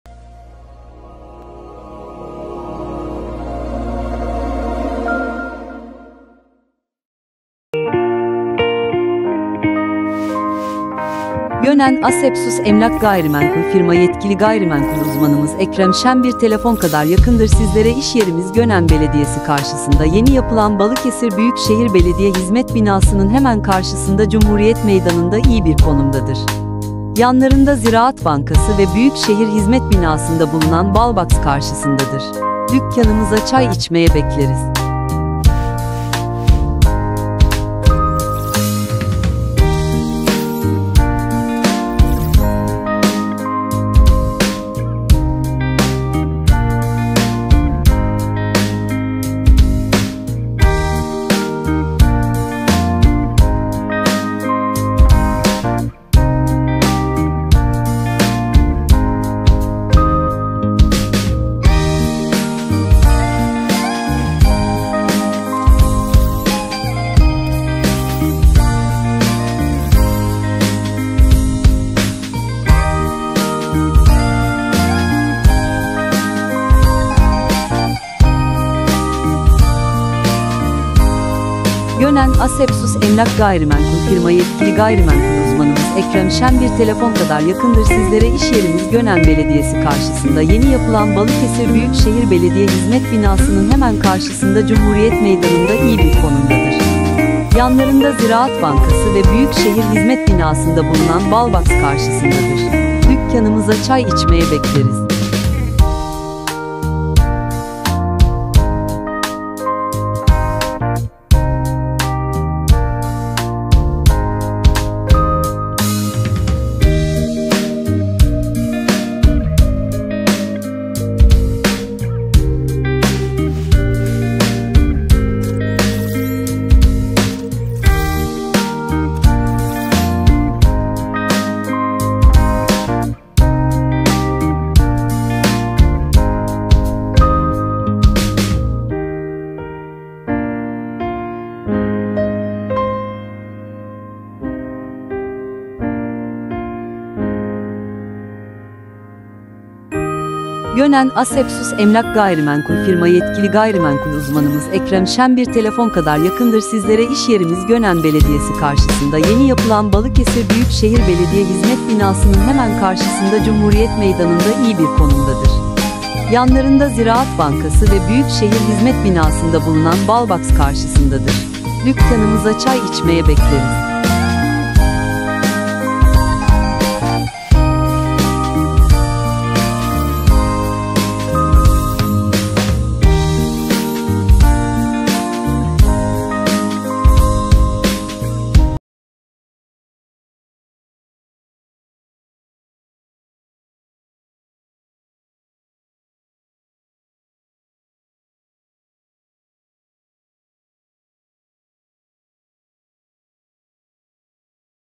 85 M2 (2+1) Yüksek Giriş Gönen Kurtuluş Mah. Onar Kent'te Daire
Dairemiz Kurtuluş Mah. Onar Kent (1 Kısım A Blok) Apartmanında

4 Katlı Binamızın Yüksek Giriş Katında Yer Almaktadır.
Binanın Dış Montalama Yapılmıştır. (Isı Problemi Yoktur.)
Binamızın Önünde Ve Çevresinde Park Sorunu Yoktur.
Arka Tarafa Bahçeye Bakan Kısımdadır.
85 M2 (2+1) Kullanım Alanına Sahiptir.
Banyo Ve Ayrı Tuvalet Vardır.
Baymak Nova Dens Kombi Takılmıştır. (Doğalgazlı)
Giriş Özel Dolaplar İle Formanto Yapılmıştır.
Gömme Dolaplar Vardır Girişte
Orta Okul Ve Lise 3-5 Dakika Yürüme Mesafesinde
Karşı Tarafta A-101 Market Vardır.
Bahçesi Ve 2 Adet Balkon Vardır.

Ticari Taşınmaz Yetki No: 1000920
GÖNEN ASEPSUS EMLAK & GAYRİMENKUL - EKREM ŞEN

AKÇAALİ MAHALLESİ CUMHURİYET MEYDANİ NO 26 / B
GÖNEN/ BALIKESİR